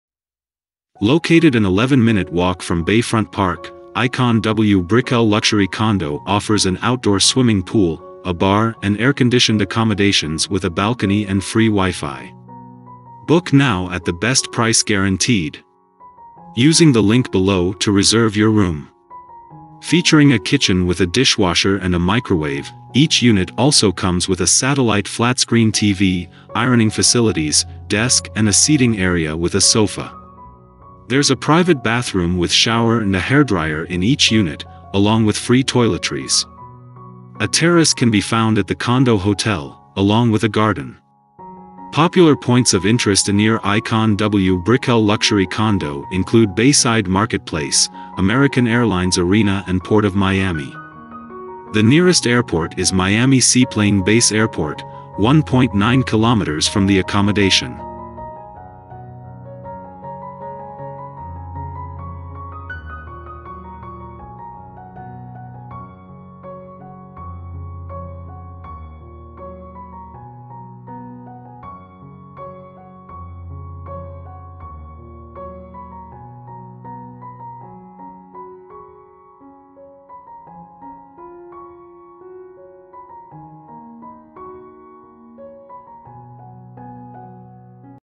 ICON W BRICKELL LUXURY CONDO
Book now
Located an 11-minute walk from Bayfront Park, ICON W BRICKELL LUXURY CONDO offers an outdoor swimming pool, a bar and air-conditioned accommodations with a balcony and free WiFi.

Featuring a kitchen with a dishwasher and a microwave, each unit also comes with a satellite flat-screen TV, ironing facilities, desk and a seating area with a sofa. There's a private bathroom with shower and a hairdryer in each unit, along with free toiletries.

A terrace can be found at the condo hotel, along with a garden.

Popular points of interest near ICON W BRICKELL LUXURY CONDO include Bayside Market Place, American Airlines Arena and Port of Miami. The nearest airport is Miami Seaplane Base Airport, 1.9 km from the accommodation.